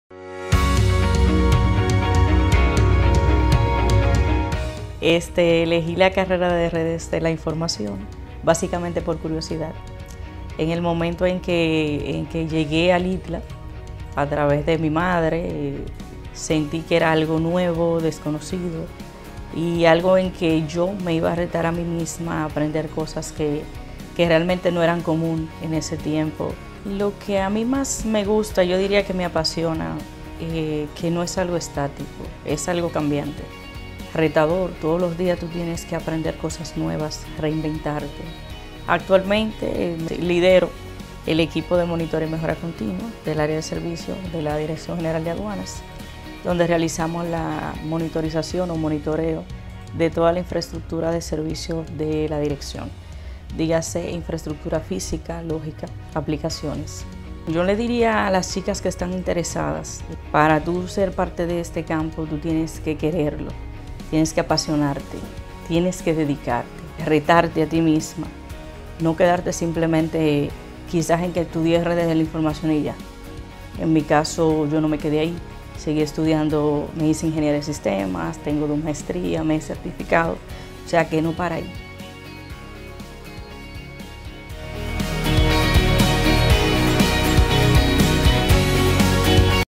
Campaña YoTambién: Thelma Feliz, Especialista en TI
¡Más tecnología! ¡Más aprendizaje! ¡Más historias! 👩🌐

«Lo que más me gusta y apasiona de la carrera en Redes de Información, es que no es algo estático, es retador, aprendes cosas nuevas... Para ser parte, debes quererlo, apasionarte, dedicarte, retarte a ti misma». ¡Uff! a Thelma se le nota que es una apasionada de la tecnología 😍 y pensar que inició este recorrido "básicamente por curiosidad" 😌 y claro, con el apoyo de su madre. 🥰

Ella dijo YoTambién🎓 y se capacitó en el área tecnológica de su elección. Así que tú también puedes 📚

Queremos sumar MásMujeresEnlaTecnología ¿Te animas? 🎬